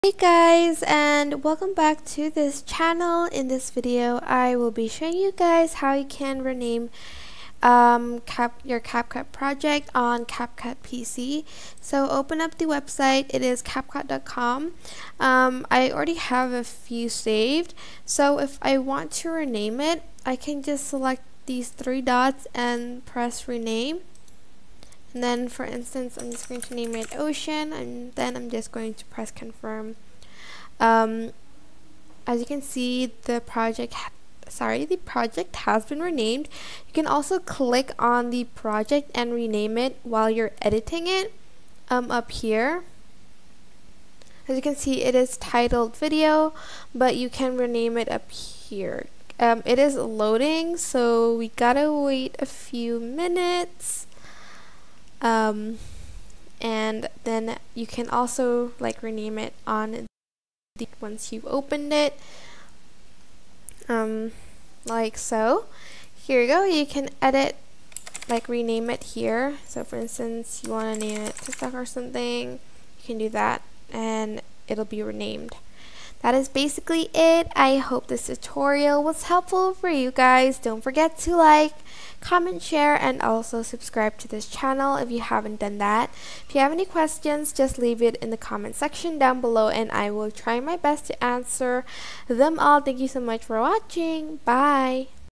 How To Rename Projects On CapCut PC
Want to rename projects on CapCut PC? Here's how you can do that.